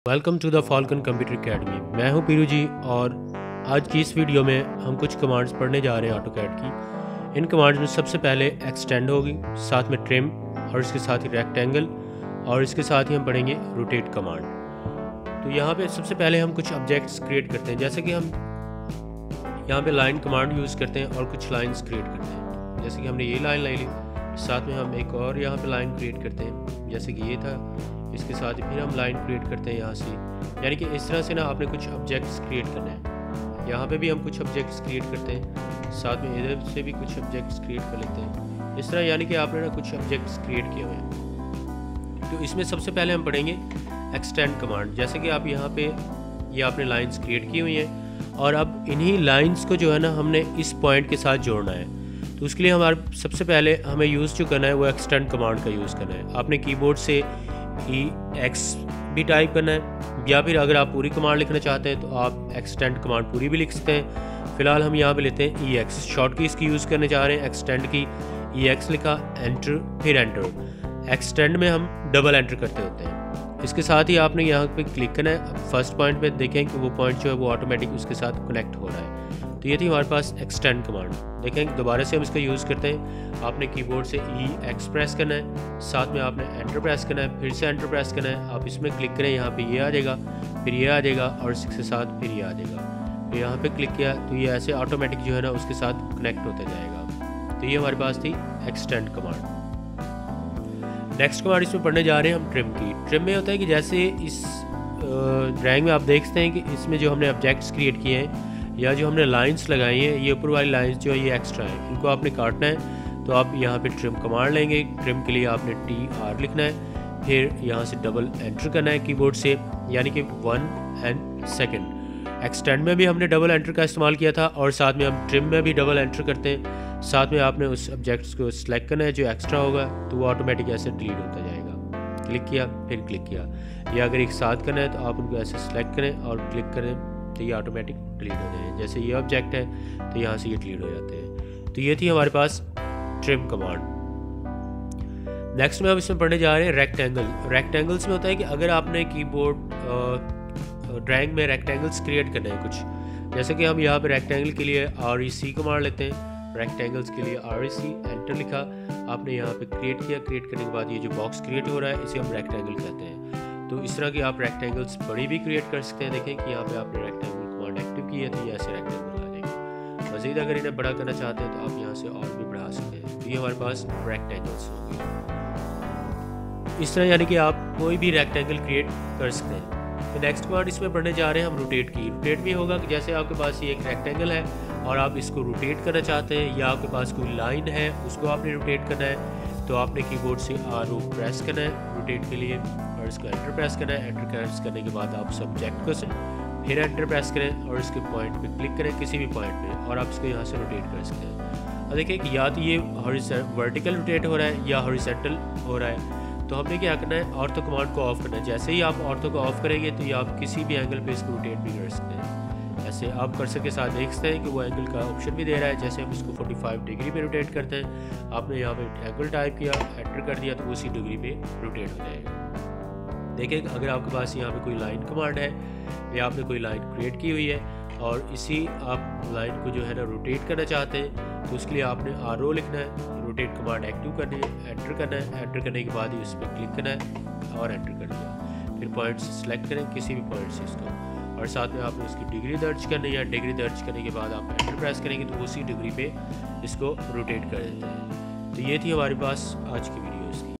Autocad training in urdu II hindi II Class 4 step by step II extend II trim II rectangle II rotate
Welcome to falcon computer academy,
This is 2nd video for Beginners of AutoCAD learn.
after watching video.

Peerunaike

Peerunaike